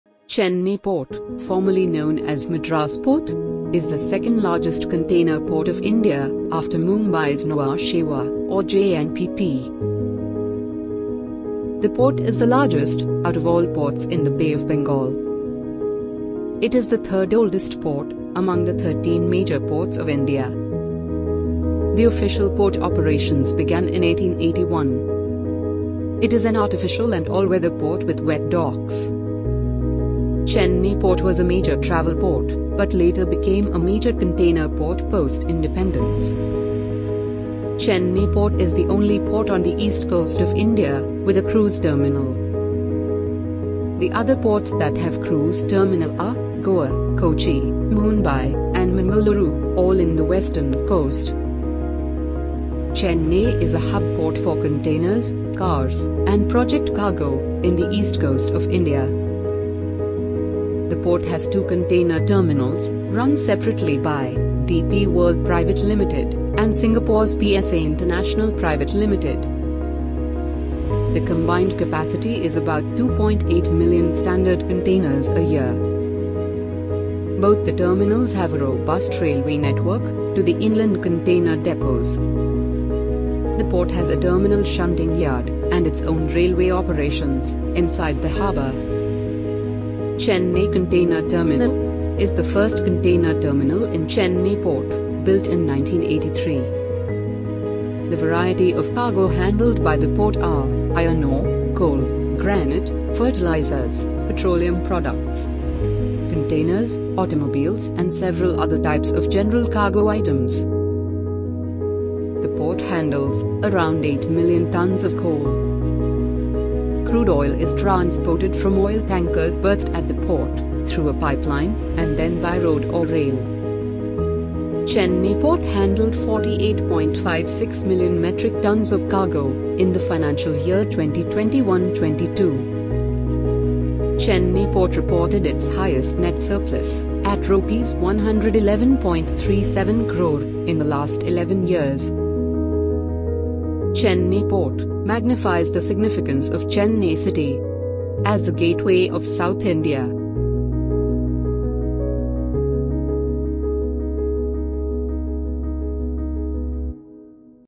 Ports of India- Chennai Port
Chennai Port, formerly known as Madras Port, is the second largest container port of India, after Mumbai's Nhava Sheva or JNPT.
Chennai Port is the only port on the East Coast of India with a cruise terminal.
Chennai Port reported its highest net surplus at Rs 111.37 crore in the last 11 years
Chennai port magnifies the significance Chennai city, as the Gateway of South India.